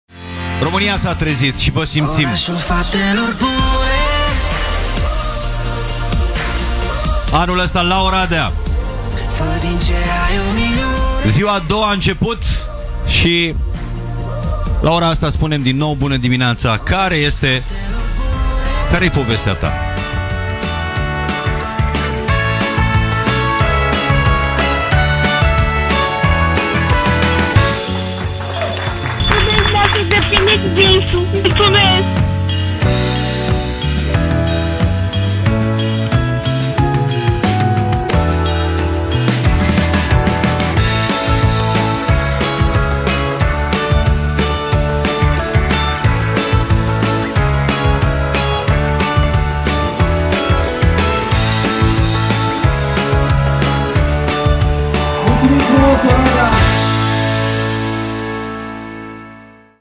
Orașul Faptelor Bune 2019 - A doua zi
Opt DJ-i s-au mutat pentru o săptămână în Casa Radio ZU din Orașul Faptelor Bune. Misiunea lor: să transforme cât mai multe visuri în realitate. Uite cum a fost doua zi în 60 de secunde!

Transmitem live 24/24 tot ce se întâmplă în a 4-a ediție a Orașului Faptelor Bune pe